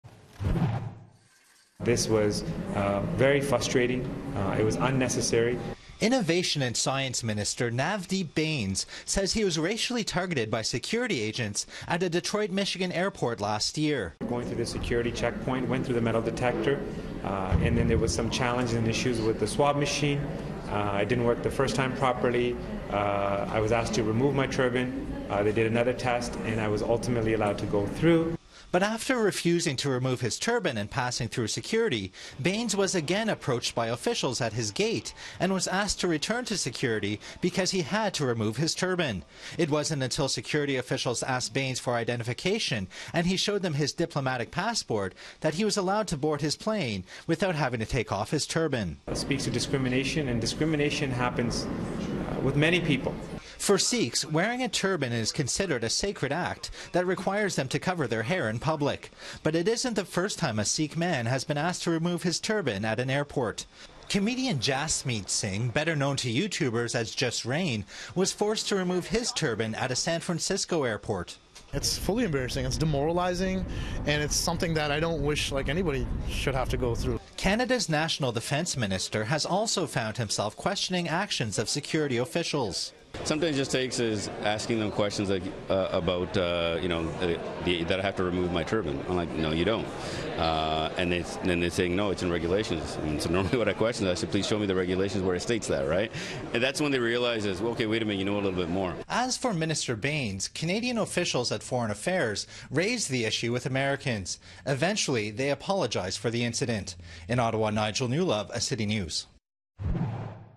Minister says he was asked to remove turban at U.S. airport
Innovation Minister Navdeep Bains says he was asked to remove his turban after a problem at a security checkpoint in a Detroit airport last year. Nigel Newlove reports.